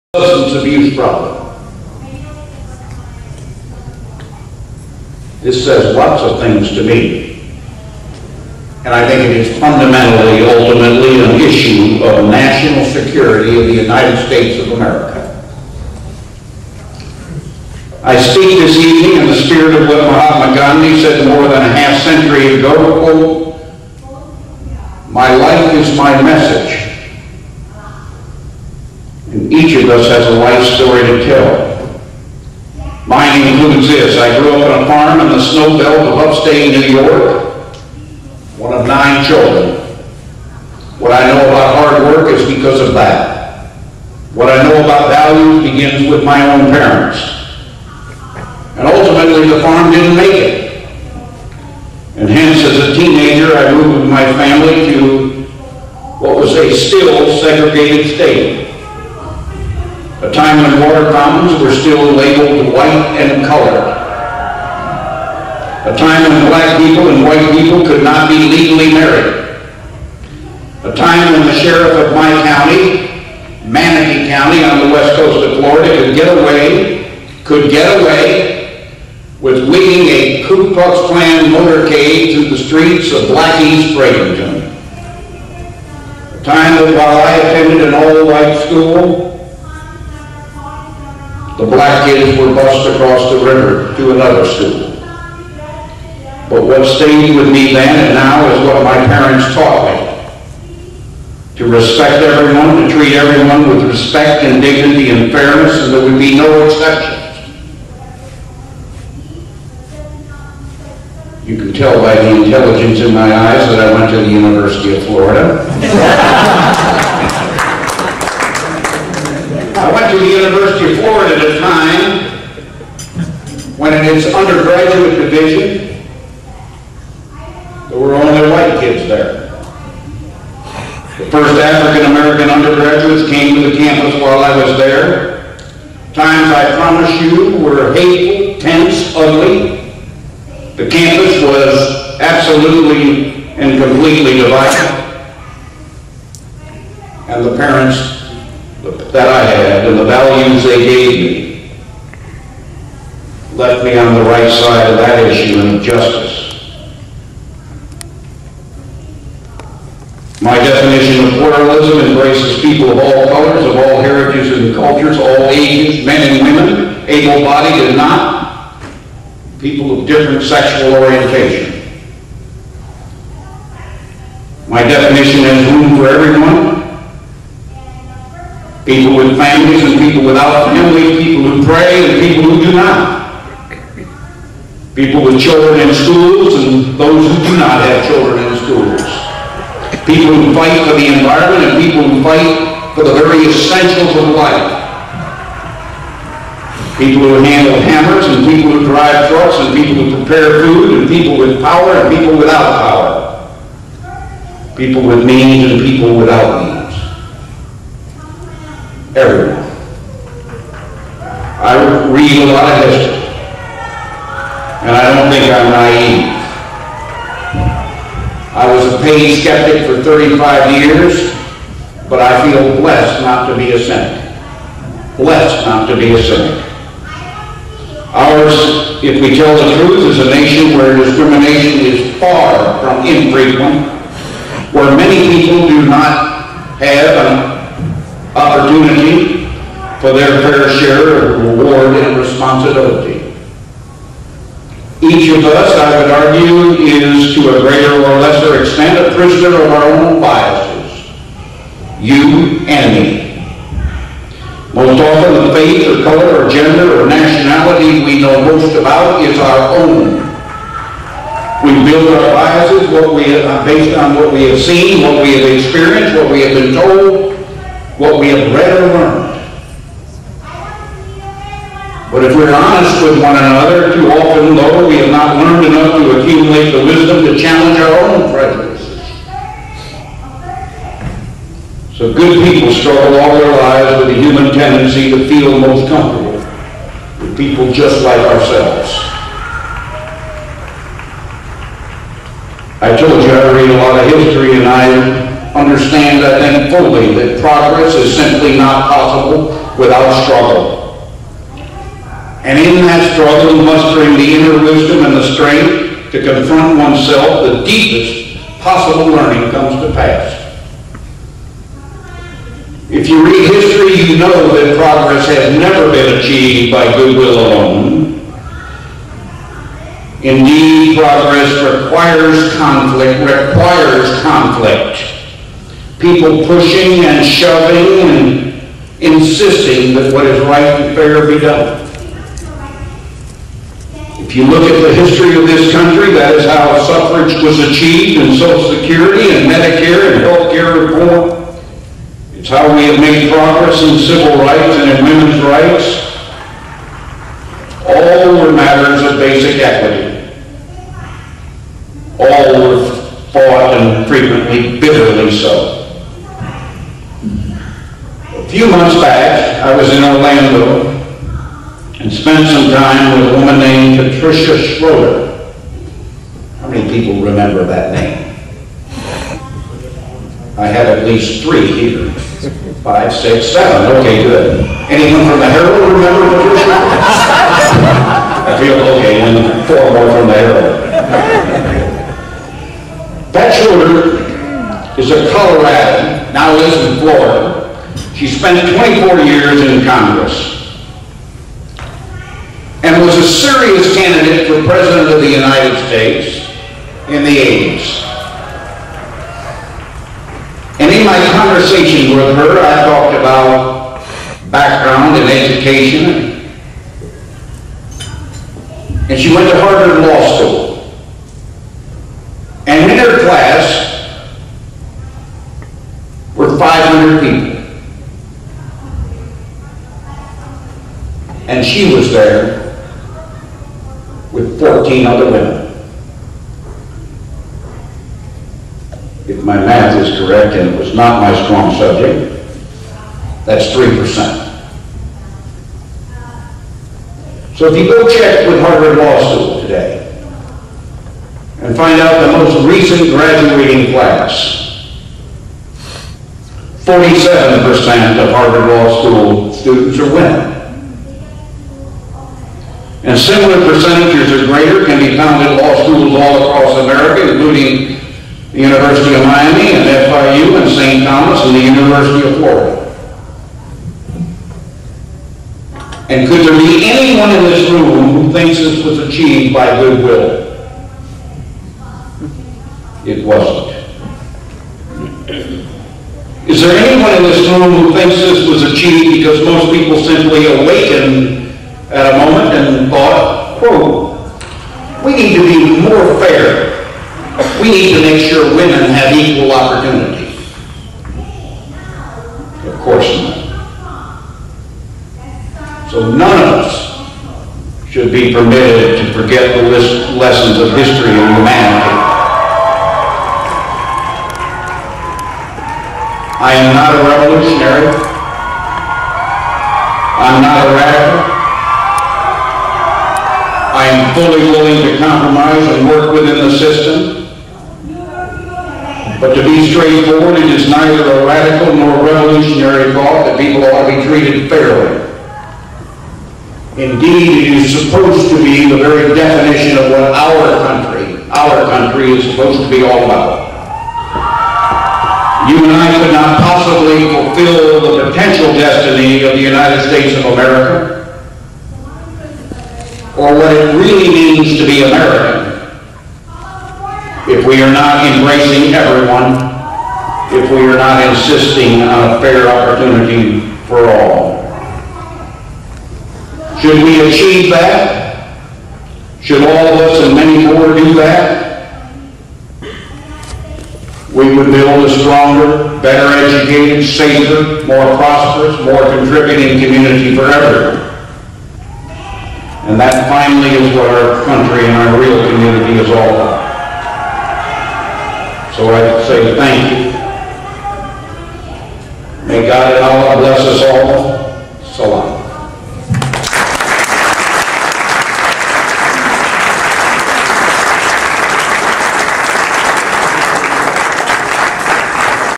David Lawrence Jr. (Keynote Speaker) Former Publisher Miami Herald Media Company 2/2
COSMOS - Coalition Of South Florida Muslim Organizations